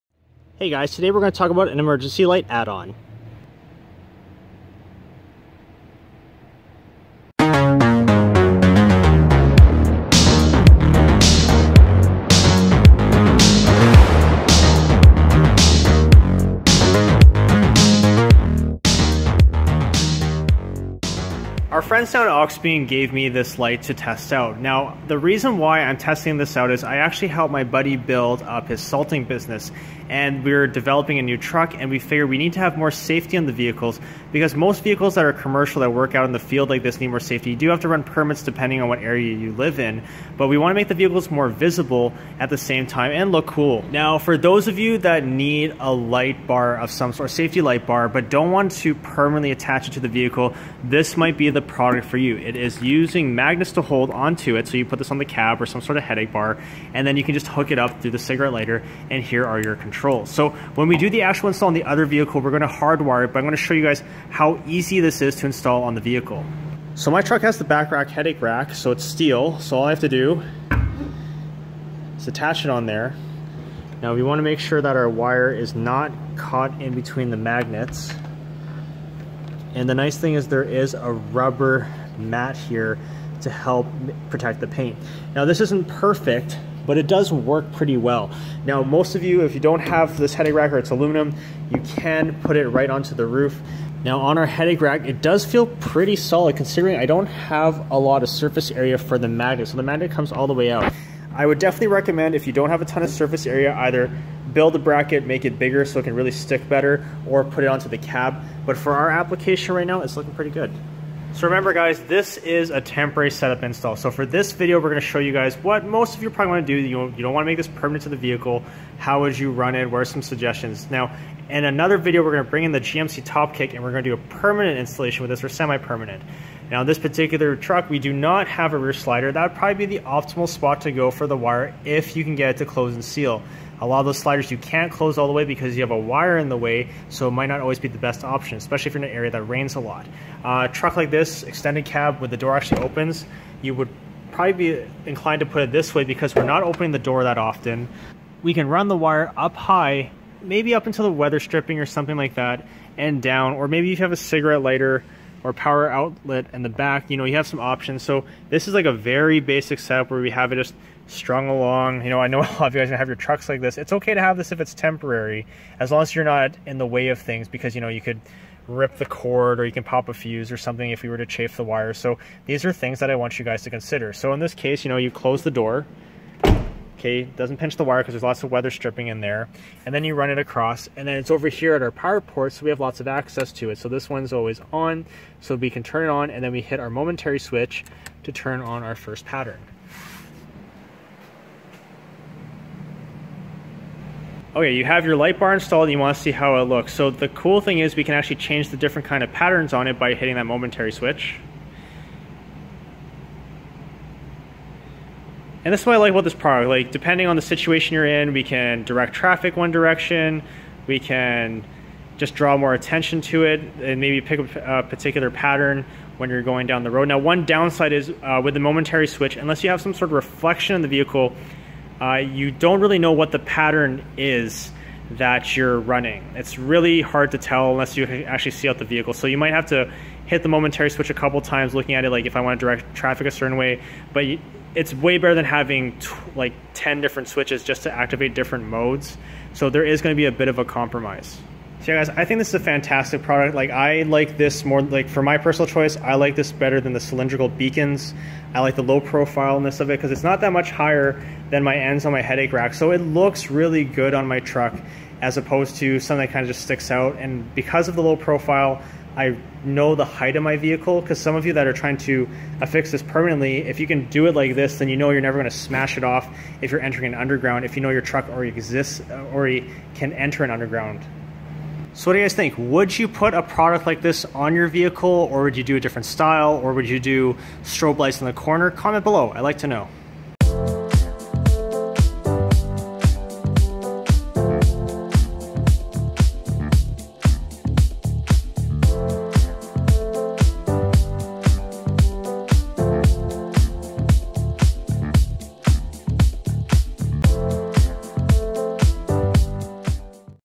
Auxbeam 21 Inch LED Emergency Strobe Light Hazard Warning Light Bar | AnthonyJ350
We try out AUXBEAM's Super Bright- High Intensity LED with 40W of power with 360 degrees of coverage, you can expect everyone in your surroundings to be alerted.

Features:
-14 selectable flash patterns
-Easy to on/off cigarette lighter plug
- 2nd button changes the strobe patterns and it memorizes the last pattern used.

Material of mini bar:
- Amber light bar exterior frame is made of double-layer aluminum alloy board.
-The base has 4 heavy duty magnetic mounts and protective rubber feet.
-14 Flash Patterns - Memory recall - Choose from a variety of warning patterns and steady burn mode.

Application: Great for emergency workers, postal service, construction vehicle, security Hazard warning, and other emergency needs.

About AnthonyJ350

Canada